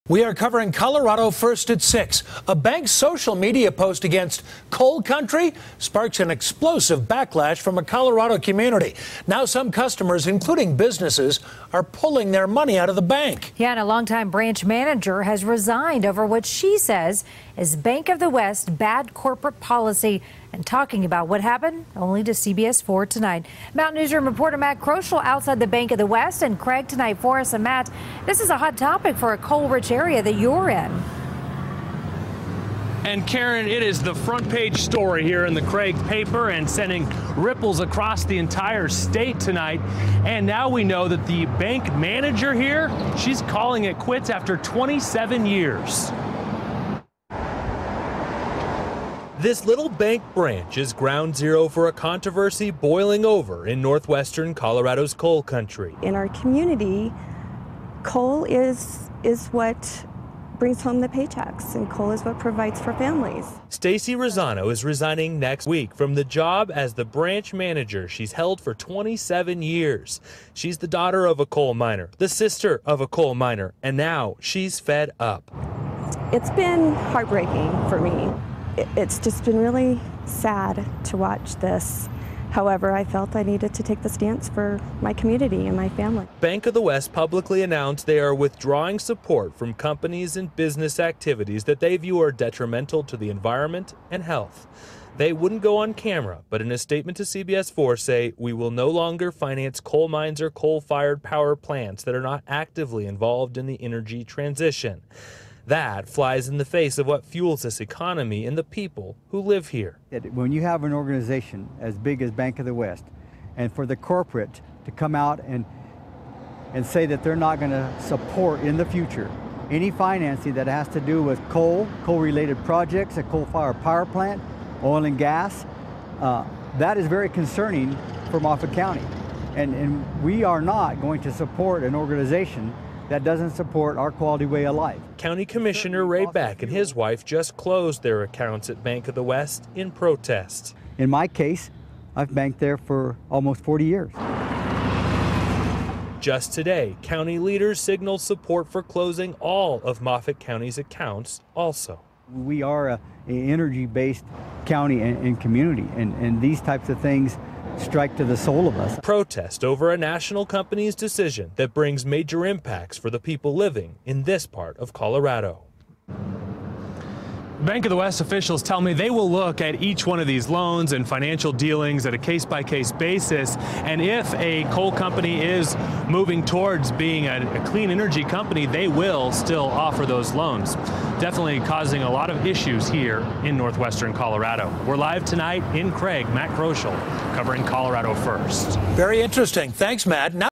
Bank Of The West Changes Policy On Coal Business
Some employees are leaving the bank over the new policy.